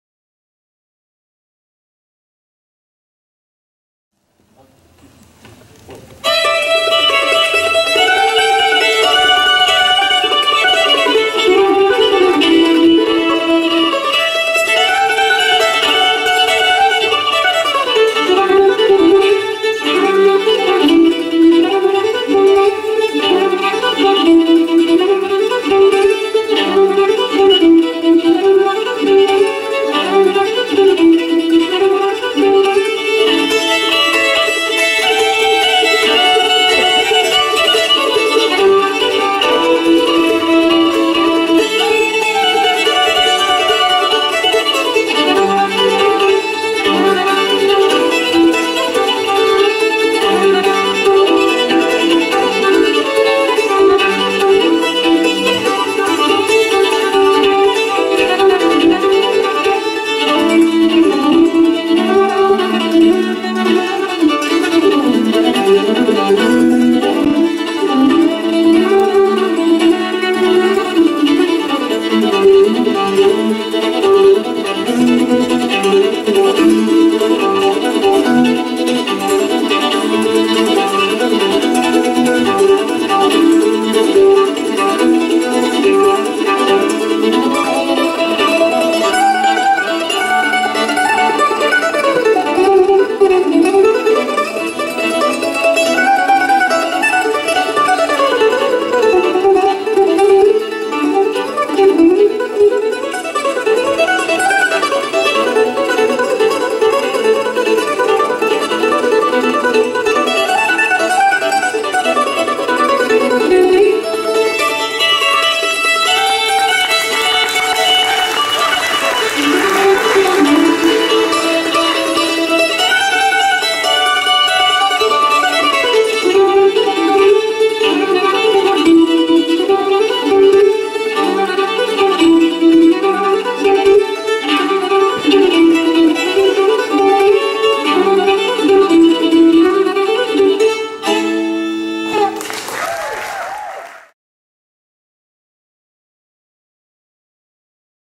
Steve Kaufman's Kamp presents John Reischman with Annie Staninec
This was recorded at the 2013 Steve Kaufman Acoustic Kamp where instructor and students come from all over the world to teach and learn acoustic music.